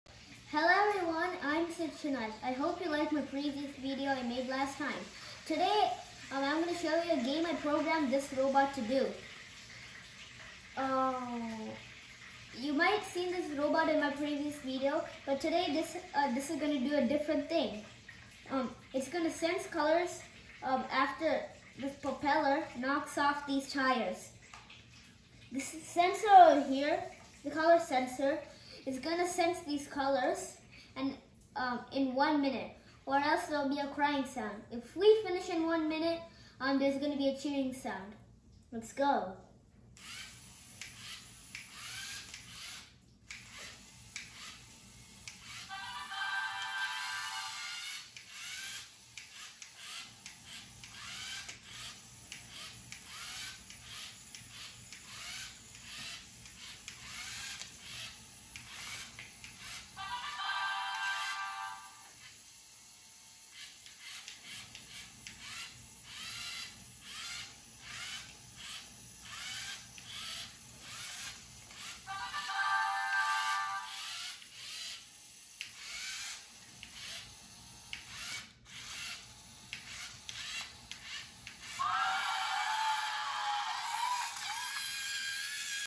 Lego Mindstorms | EV3: A Robot to sense colors
In this video, Sid Shivnath talks about how he built and programmed a Lego Mindstorms robot to sense colors and to take actions based on the colors detected.